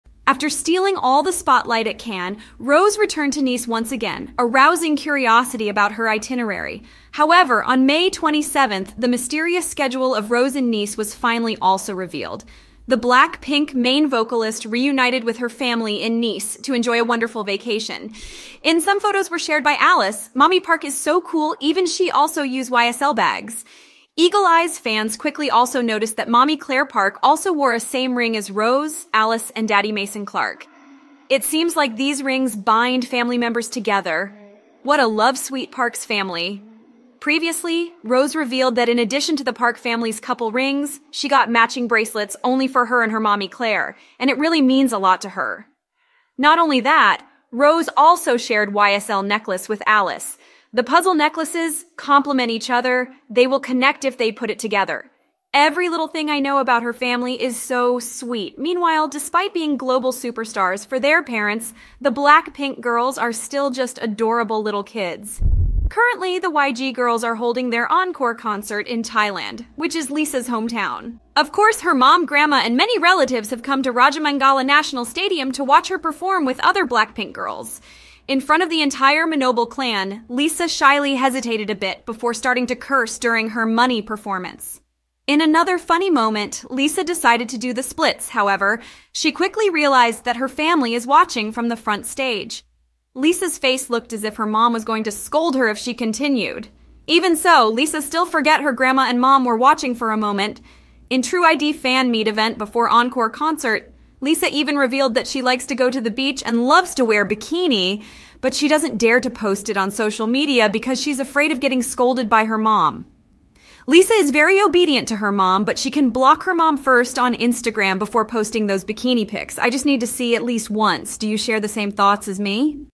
Lisa talks about her BlKlNl photos, Rosé reveals meaningful couple rings
During BLACKPINK's Born Pink Encore Concern in Thailand, Lisa talks about her BlKlNl photos in trueid event, Rosé family's meaningful couple rings were revealed.